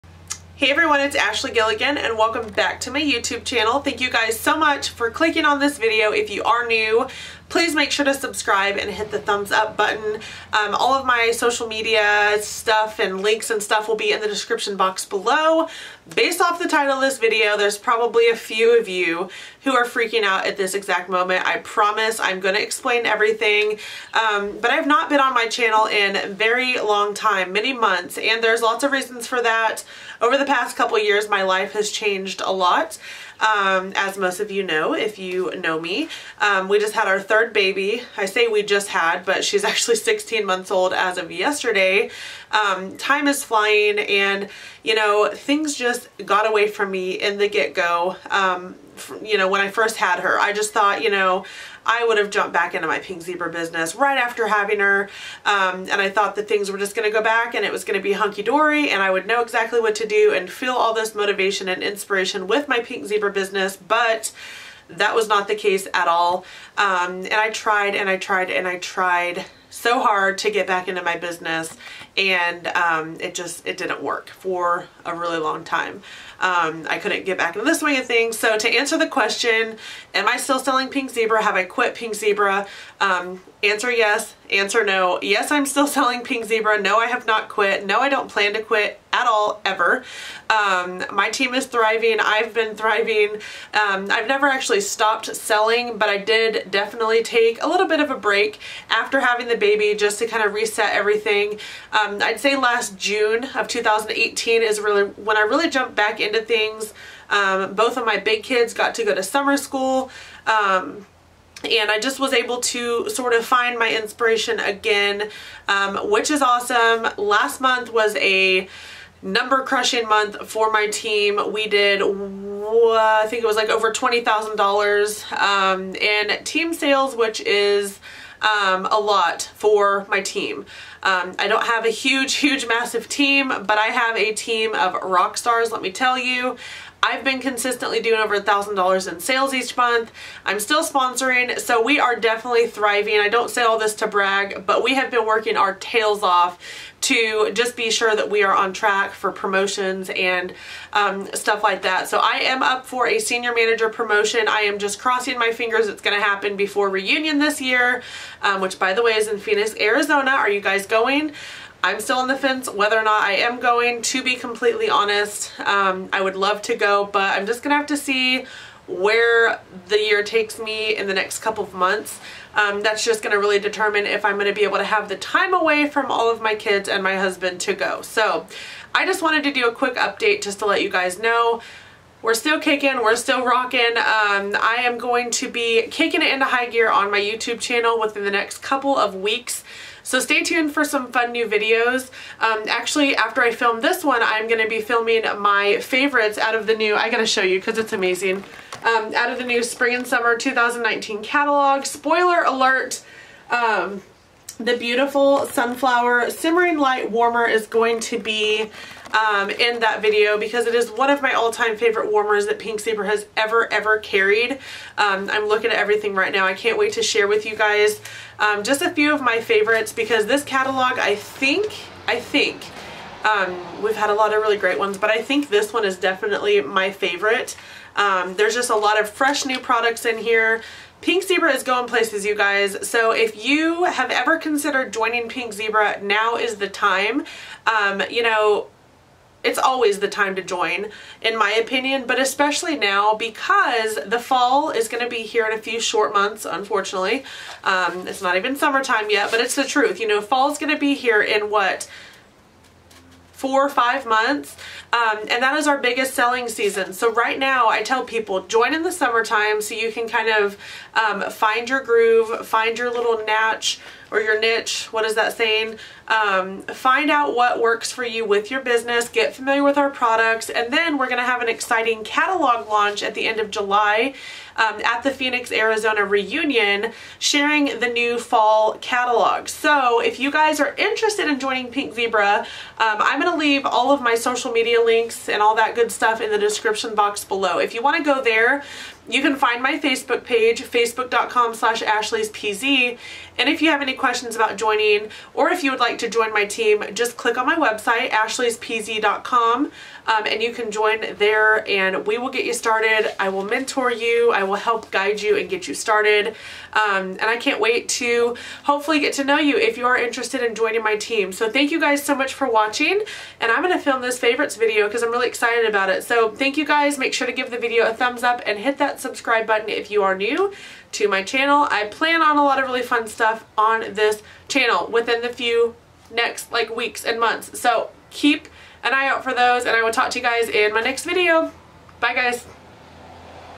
I QUIT Pink Zebra?! (Home Fragrance) | Direct Sales
Did I quit selling Pink Zebra? Find out by watching this video!
Hint: Heck no I didn't quit!!

JOIN MY TEAM!! We have fun & kick butt selling the BEST home fragrance on the market.

My VIP group- Facebook.com/groups/ashleyspz